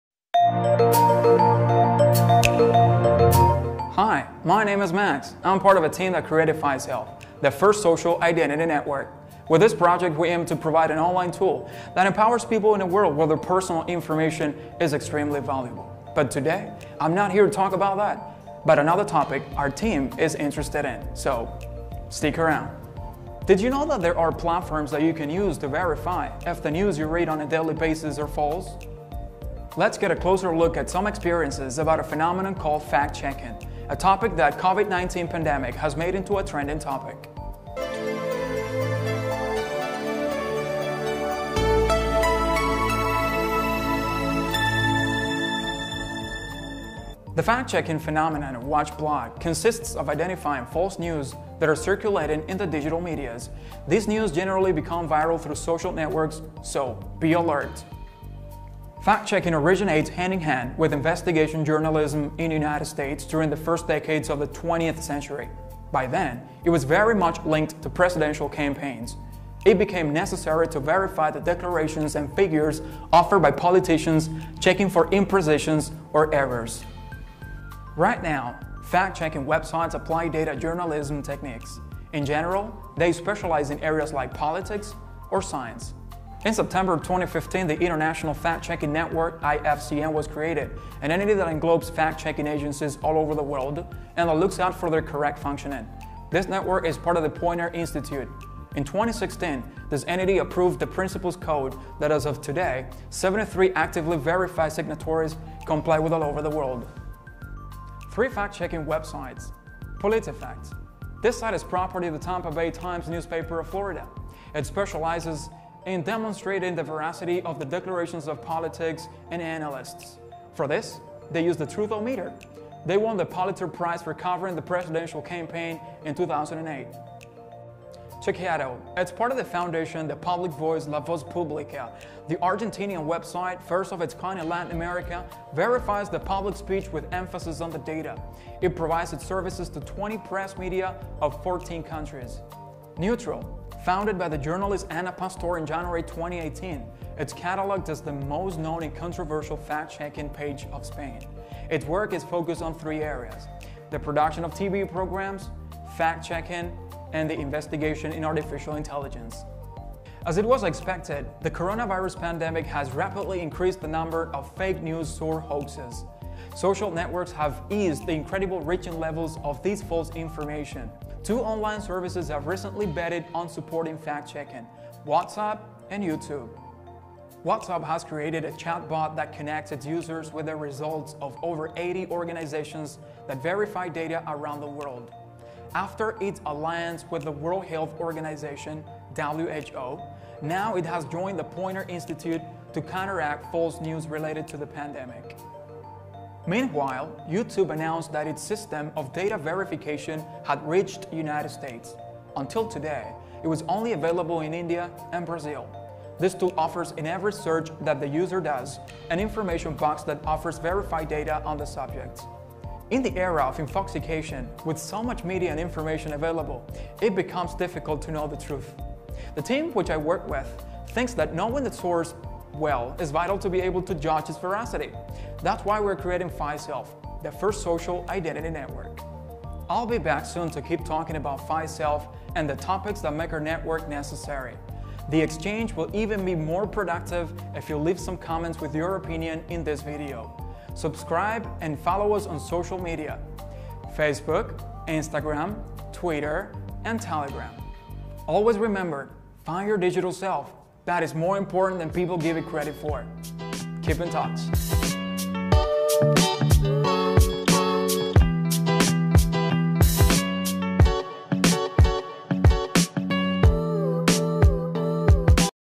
Do you know about Fact Checking? | Find your digital self - FySelf Vlogs
It is very easy to spread false or wrong information in a video or post online! We prove it to you:
In the video, we talk about the origins of factcheking, starting from the beginning of the 20th century. Actually, that phenomenon occurred at the beginning of this 21st century, as indicated by recent photos of presidential campaigns in the United States.
Hence the importance of the practice of fact checking in this infoxicated society.
On this subject you can find actually correct information on our blog 😉.